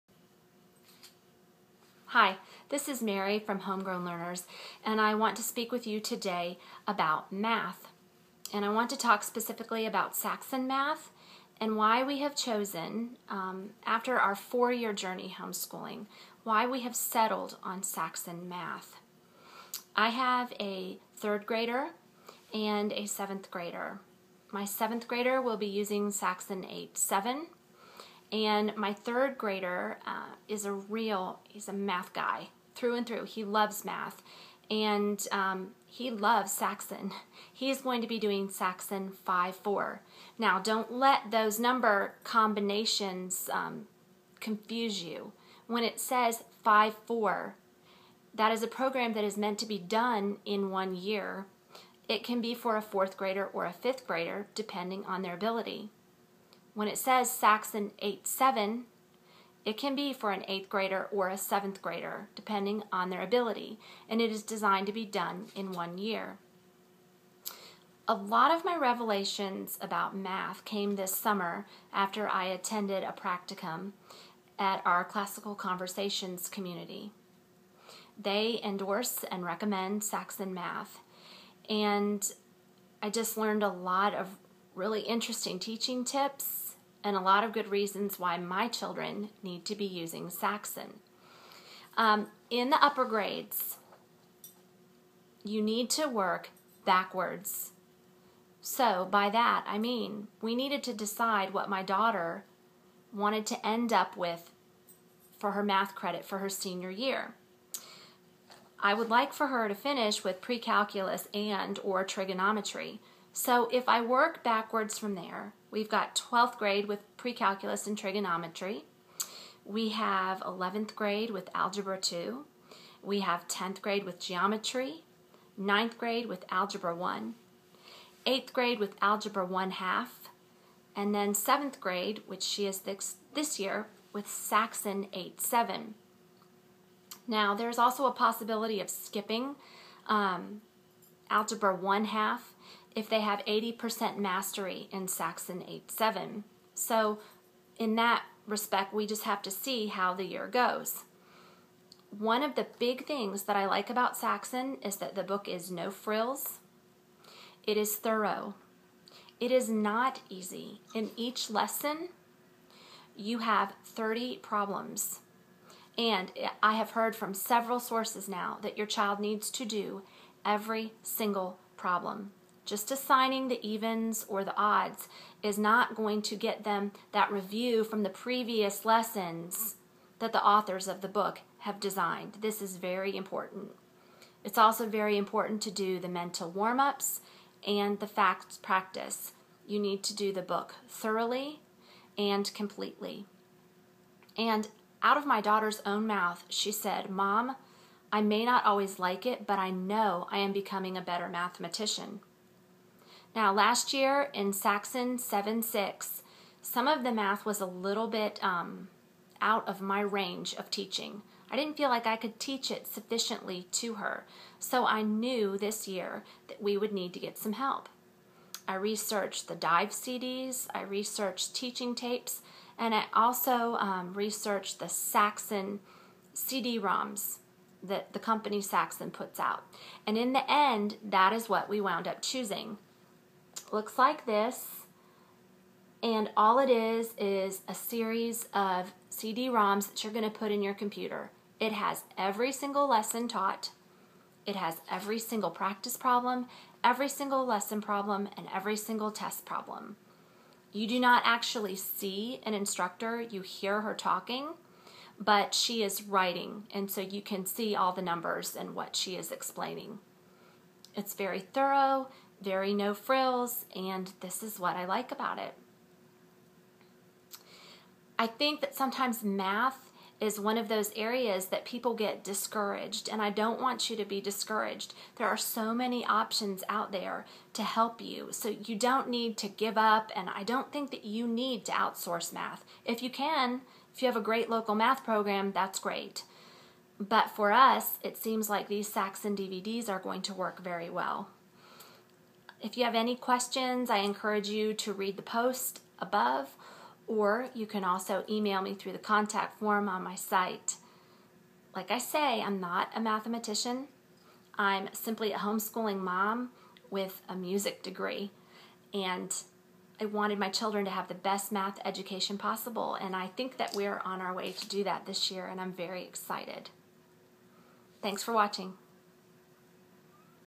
Why we use Saxon Math in our Homeschool
A description of our Saxon math choices for this homeschool year, including a review of Saxon Teacher.